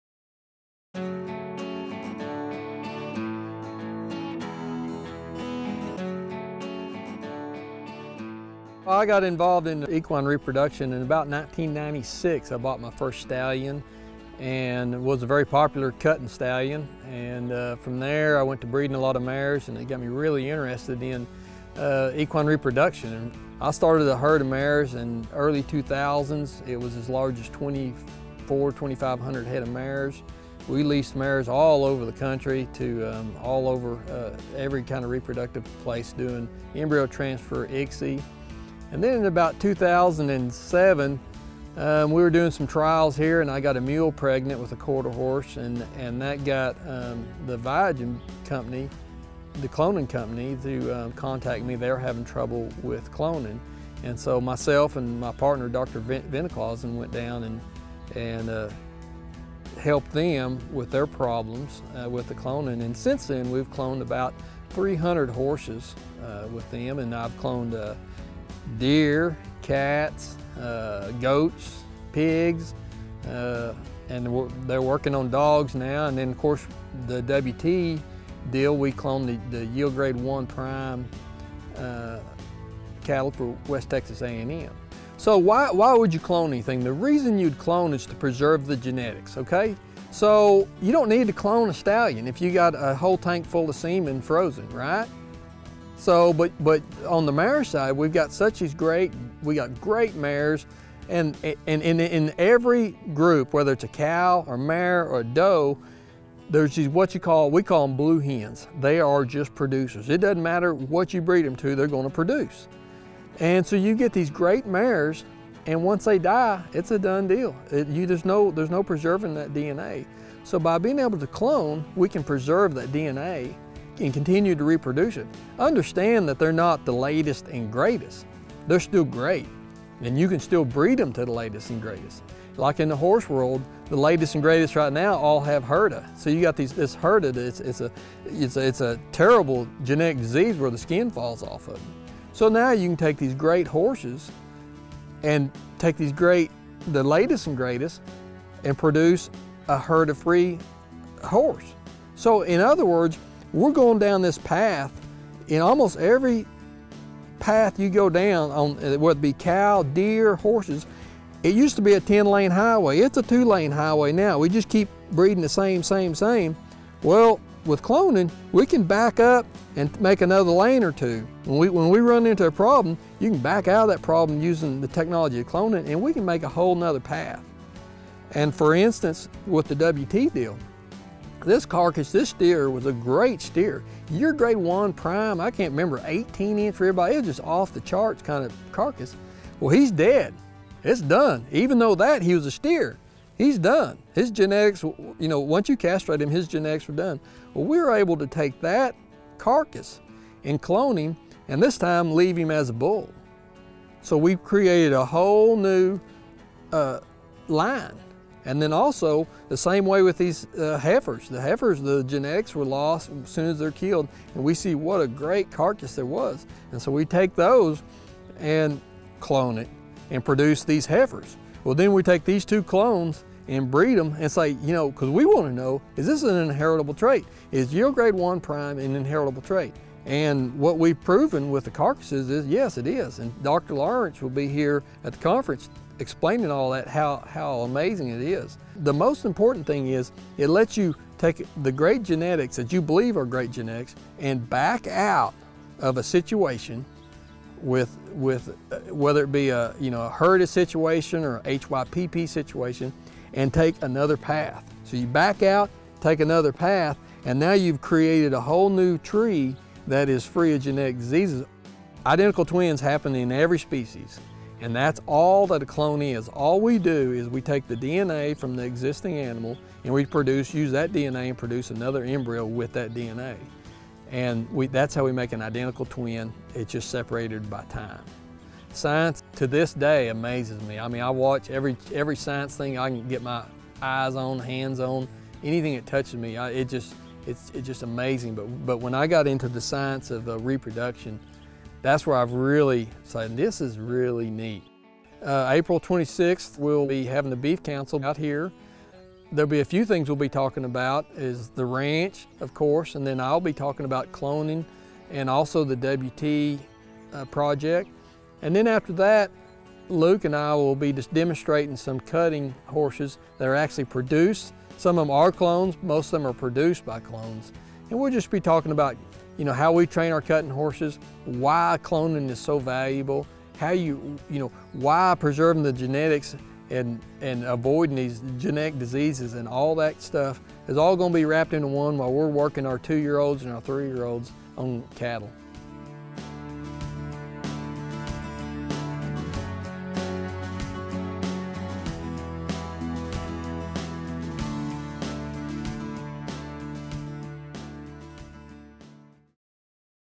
Jason Abraham: 2017 Hemphill County Field Day Host
The Mendota Ranch of Canadian, Texas will be one the field day hosts for the 2017 Hemphill County Beef Conference. Jason Abraham welcomes visitors to his unique operation that has made impressive strides in cloning technology.